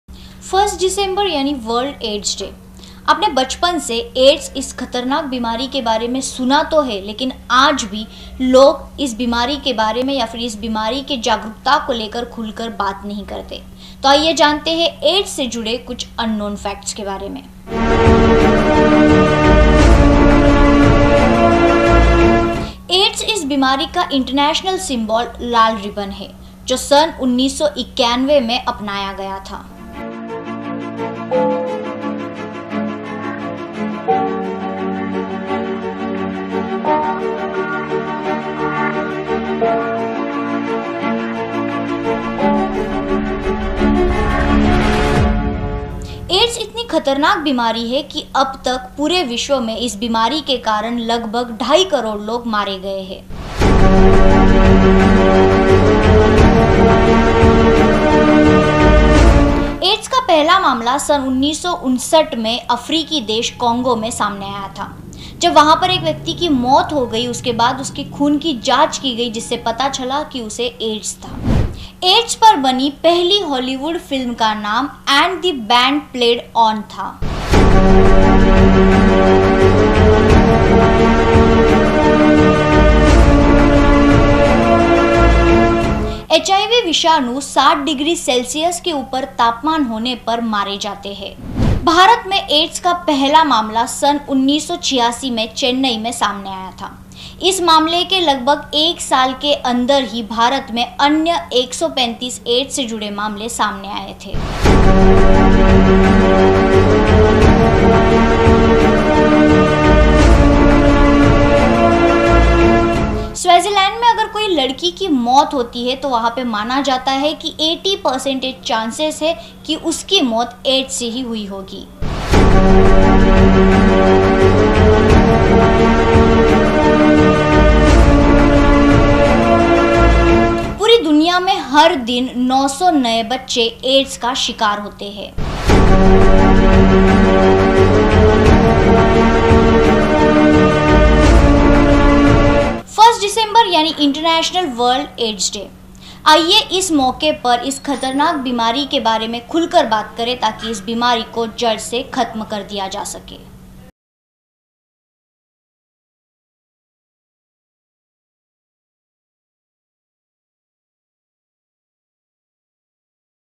World AIDS Day l 1st December (Day Special) l Manch TV l Meghna Ghag
World AIDS Day 1st December (Day Special) Manch TV

World AIDS Day takes place on the 1st December each year. It’s an opportunity for people worldwide to unite in the fight against HIV, to show support for people living with HIV, and to commemorate those who have died from an AIDS-related illness. Founded in 1988, World AIDS Day was the first ever global health day.

Anchor: Meghna Ghag
Cameraman: Sonu Shinde
Editor: Buddham Jadhav